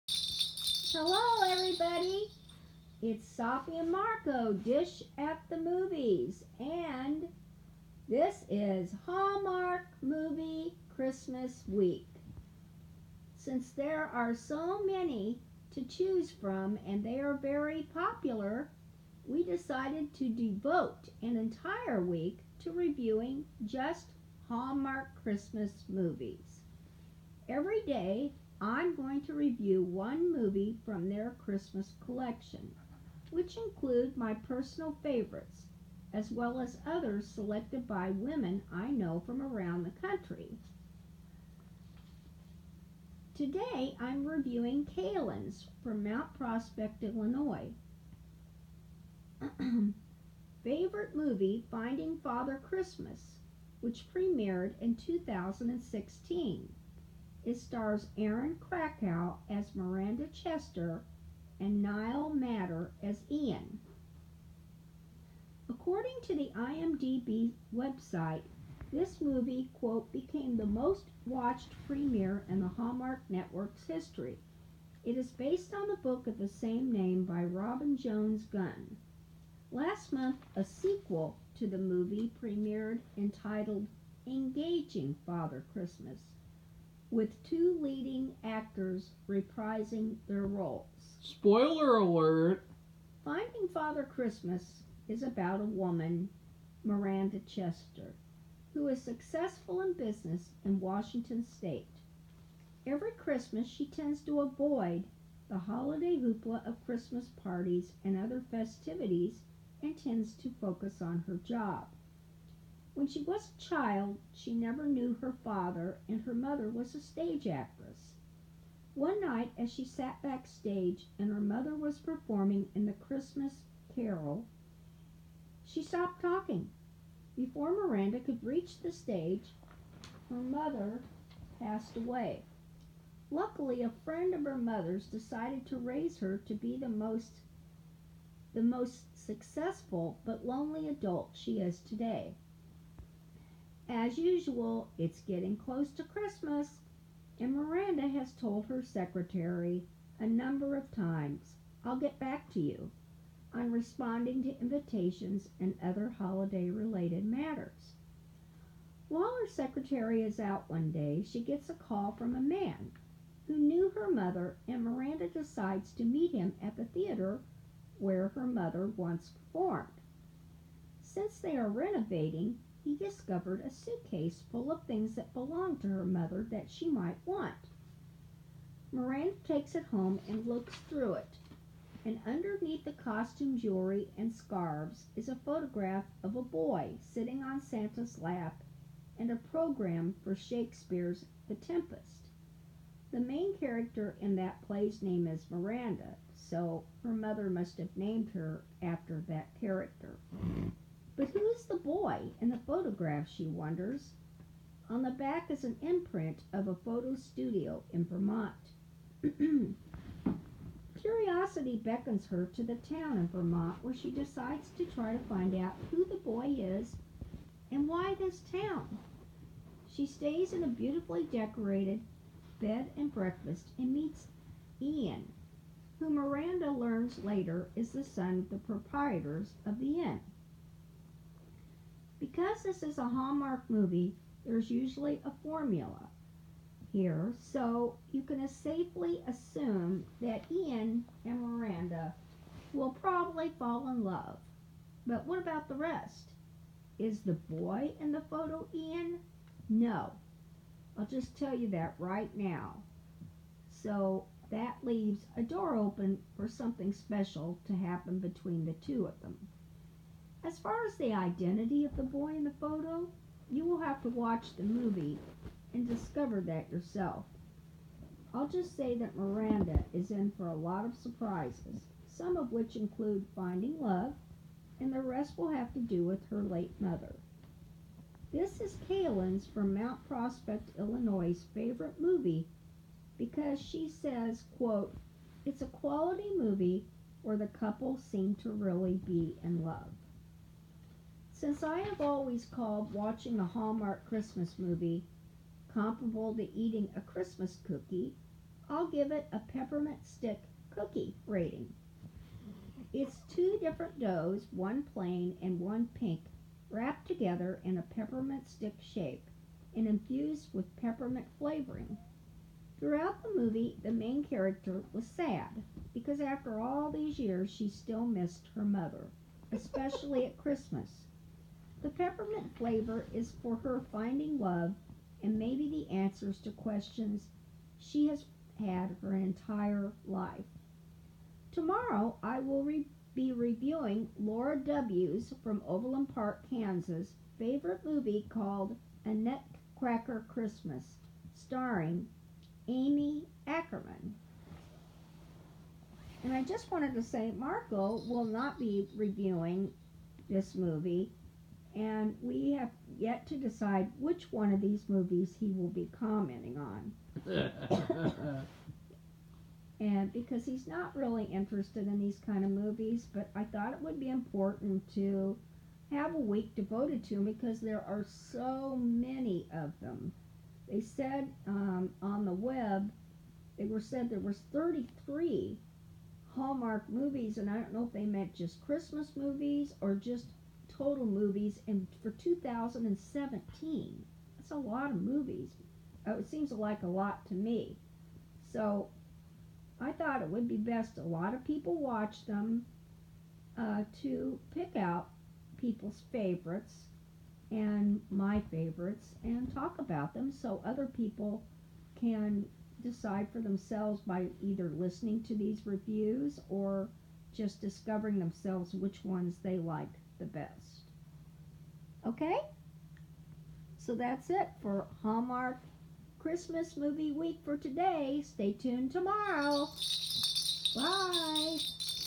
Hallmark Week Season 1 Episode 1
We review Finding Father Christmas from Kalen from Illinois.